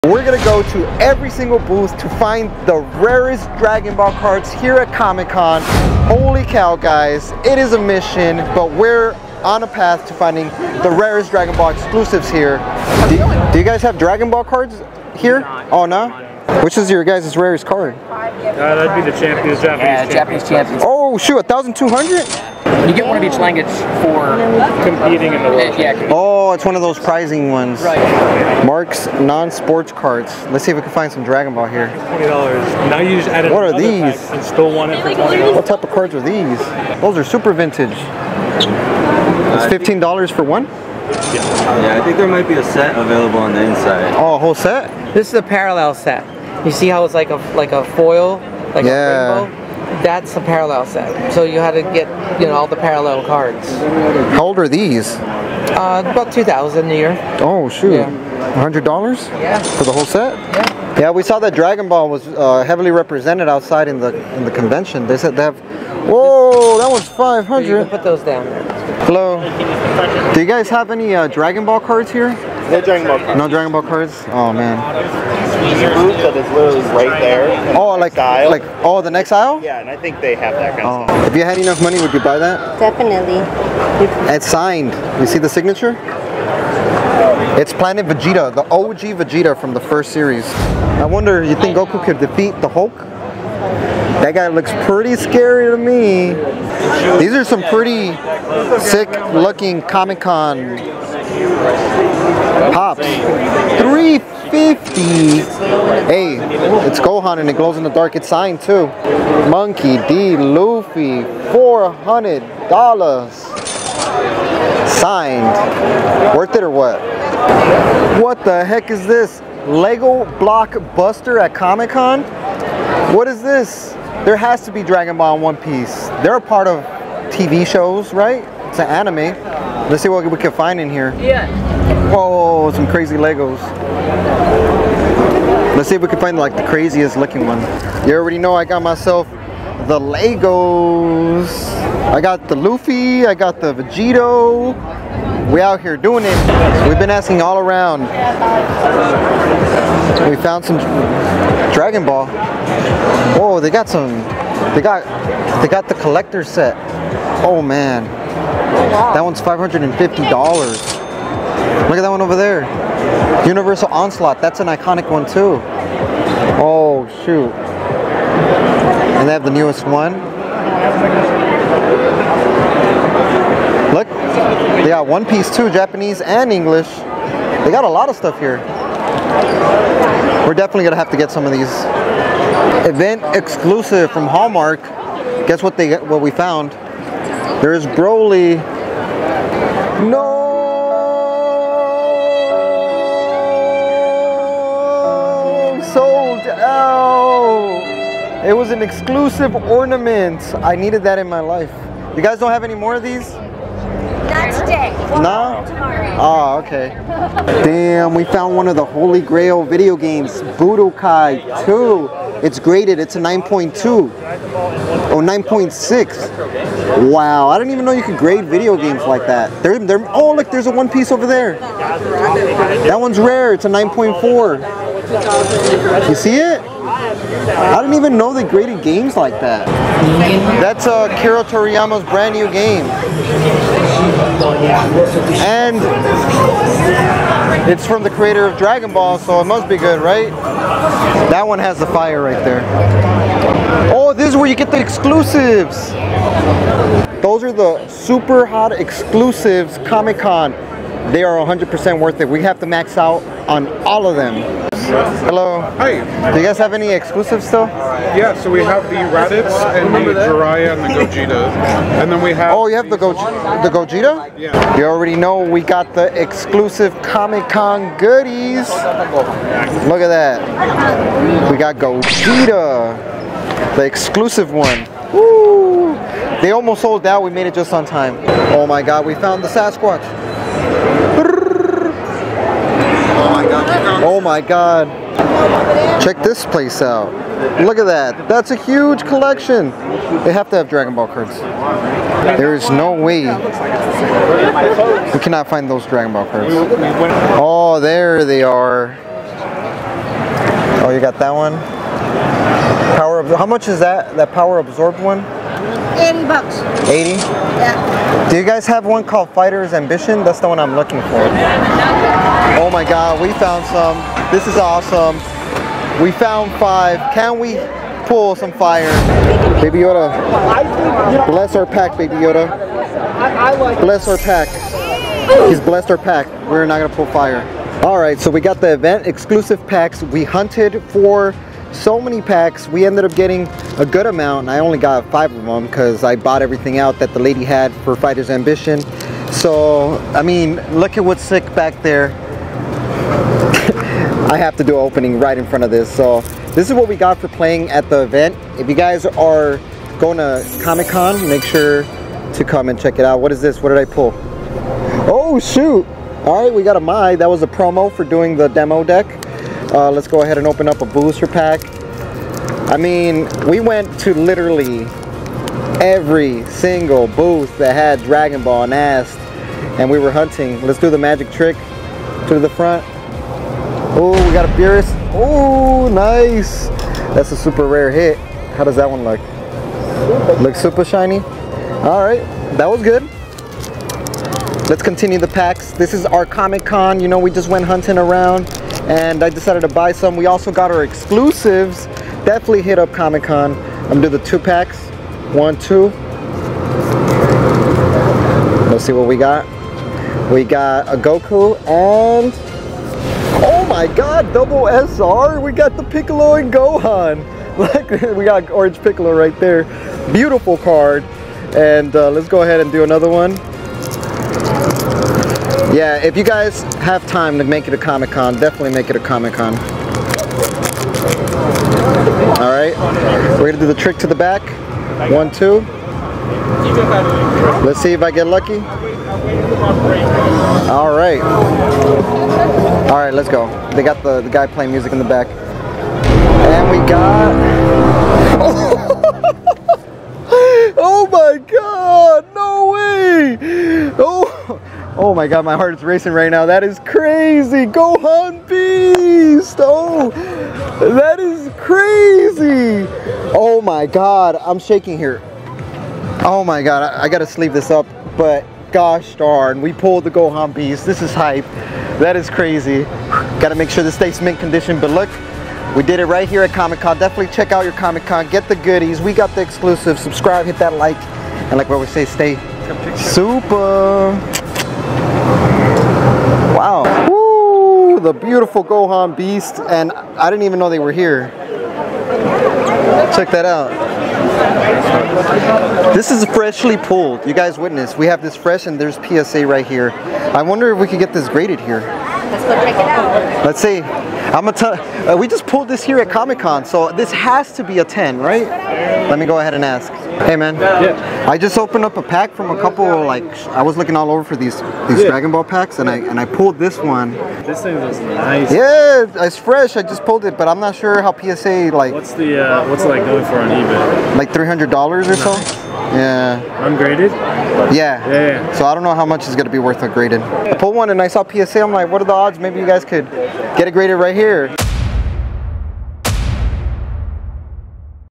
San Diego Comic Con 2023: The Rarest Gohan Beast Card Unveiled!
We went to San Diego Comic Con 2023 to find the Rare Collectable items! And in our adventure we found Gohan BEAST from Dragon Ball Super!

follow all of these to become SUPAH!